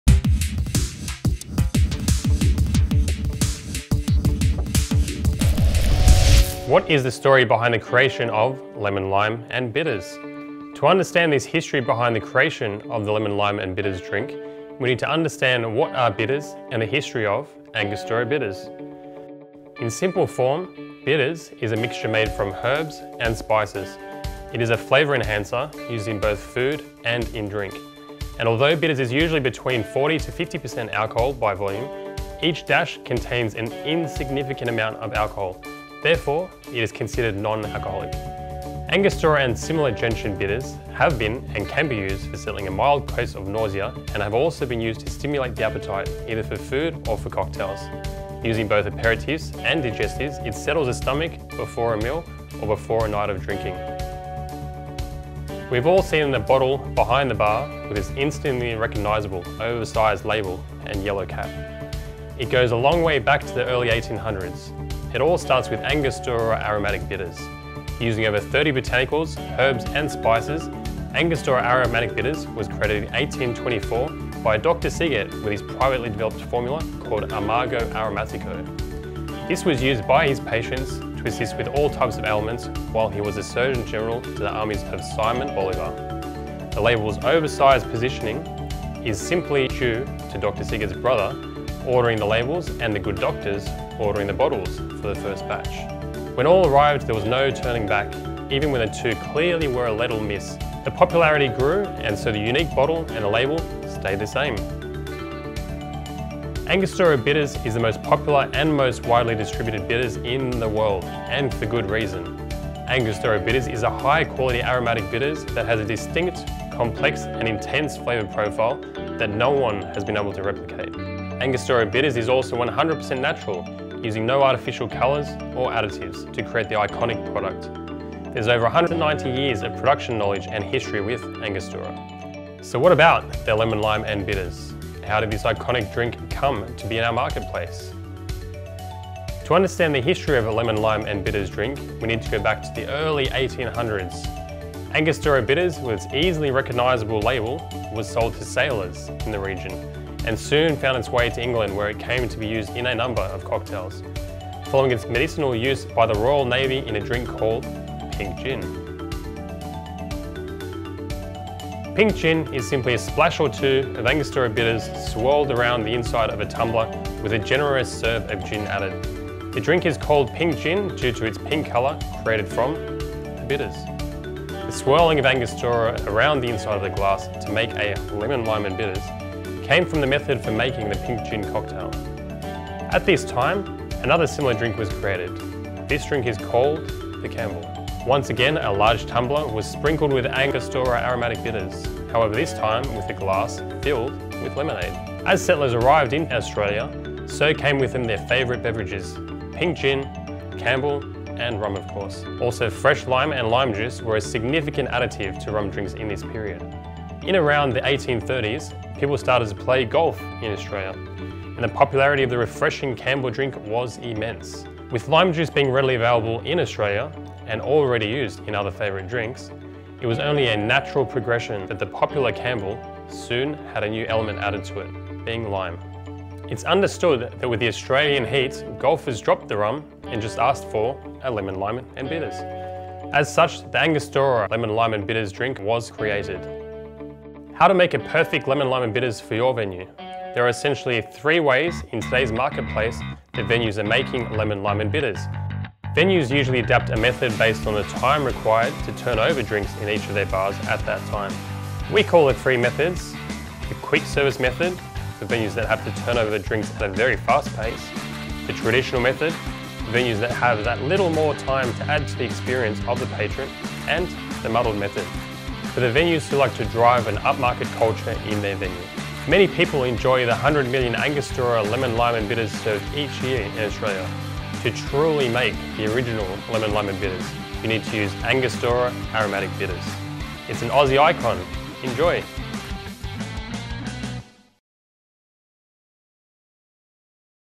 Angostura Bitters | History | Drinks Network
Angostura Bitters | History

To understand the history of the Lemon Lime and Bitters drink we need to go back to the early 1800’s…

Angostura Bitters with its easily recognisable label was sold to sailors in the region and soon found its way to England where it came to be used in a number of cocktails, following its medicinal use by the Royal Navy in a drink called Pink Gin.

Pink Gin is simply a splash or two of Angostura Bitters swirled around the inside of a tumbler with a generous serve of gin added. The drink is called Pink Gin due to its pink colour created from the bitters.

The swirling of Angostura Bitters around the inside of the glass to make a Lemon Lime and Bitters came from the method for making the Pink Gin cocktail.

At this time another similar drink was created. This drink is called the Campbell. Once again a large tumbler was “pinked” with Angostura Aromatic Bitters. However this time the glass is filled with lemonade.

As settlers arrived in Australia, so came with them their favourite beverages, Pink Gin, Campbell and rum of course. Also fresh lime and lime juice were a significant additive to Rum drinks in this time period.

In around the 1830's, people started to play golf in Australia and the popularity of the refreshing Campbell Drink was immense. With lime juice being readily available in Australia and already being used in other favourite drinks, it was only a natural progression that the popular Campbell soon had a new element added to it, being lime.

It’s understood that with the Australian heat, golfers dropped the rum and just asked for a Lemon Lime and Bitters. As such, the Angostura lemon Lime and Bitters drink was created!

Welcome to Drinks Network...where the we connect people and drinks.

MAILING ADDRESS
Drink Australia Pty Ltd T/as Drinks Network
11/81 Bertram Street, Mortlake,